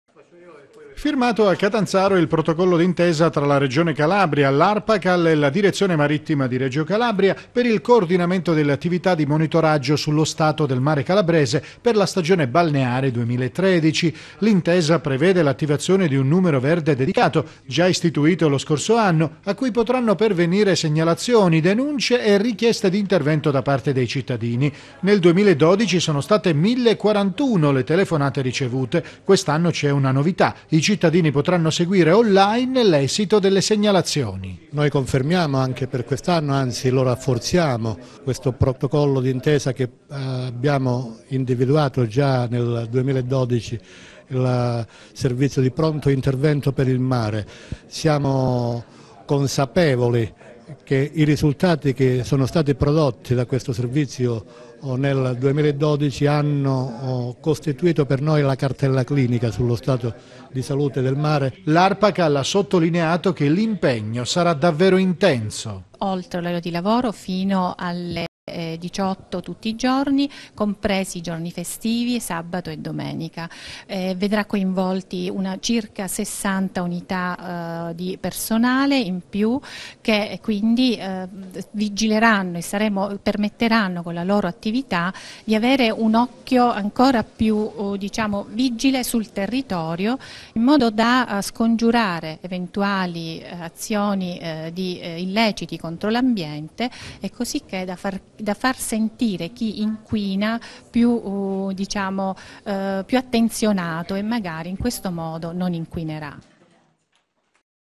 CATANZARO REGIONE CALABRIA SOS MARE
CATANZARO REGIONE CALABRIA SOS MARE FIRMA INTESA CON ARPACAL E DIREZIONE MARITTIMA PER LA STAGIONE BALNEARE 2013